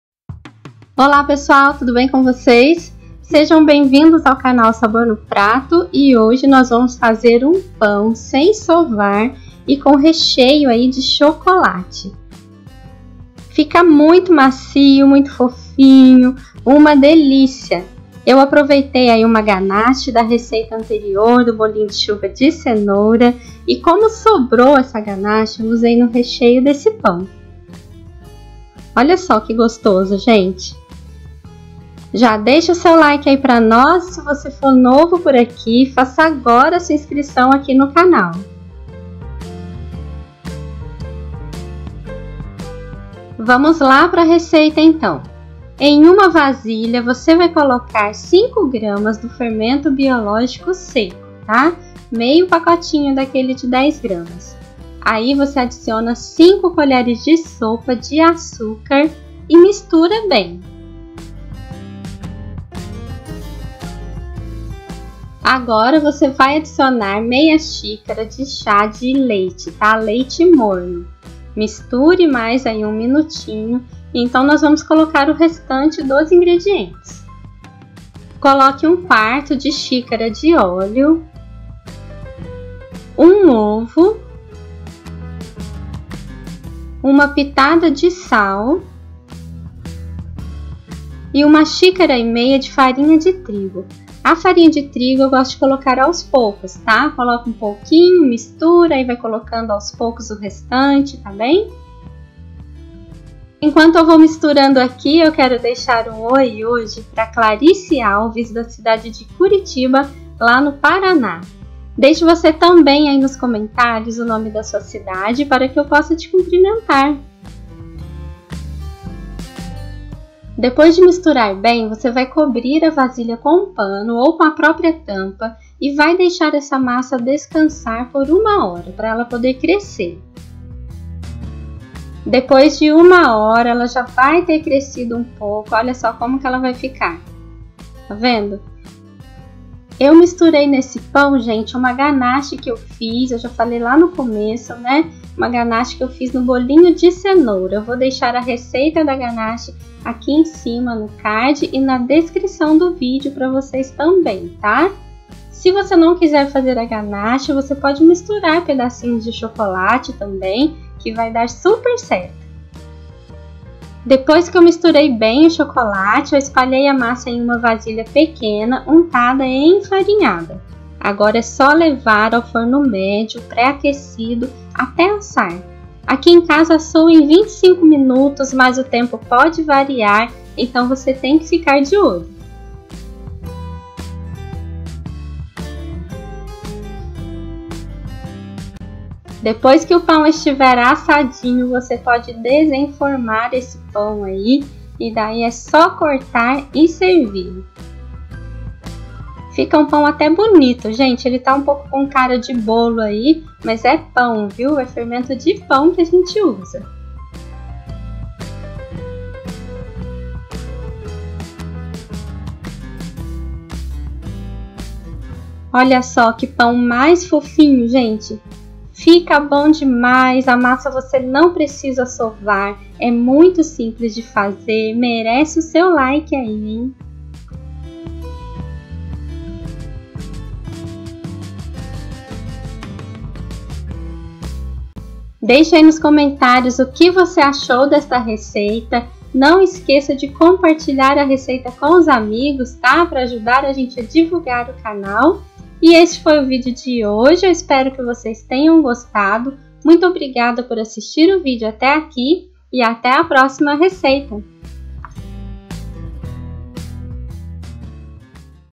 PÃO CASEIRO SEM SOVAR COM CHOCOLATE | PÃO FOFINHO E FÁCIL DE FAZER
Pão caseiro delicioso SEM SOVAR e com chocolate. Esta é uma receita fácil de um pão caseiro muito gostoso. Esta receita de pão sem sovar eu faço várias vezes aqui em casa. Faz muito sucesso e acaba rapidinho. Fica um pão muito fofinho e é muito fácil e rápido de fazer. Faça este pão sem sovar na sua casa e depois me conta, combinado? ;)

Ingredientes (xícara de 240ml):
5g de fermento biológico seco
5 colheres (sopa) de açúcar
1/2 xícara (chá) de leite morno
1/4 de xícara (chá) de óleo
1 ovo
Um pitada de sal
1 xícara e meia (chá) de farinha de trigo
Chocolate em pedaços a gosto ou uma ganache (com 100g de chocolate + 1/2 caixinha de creme de leite)

Deixe nos COMENTÁRIOS o NOME DA SUA CIDADE !!!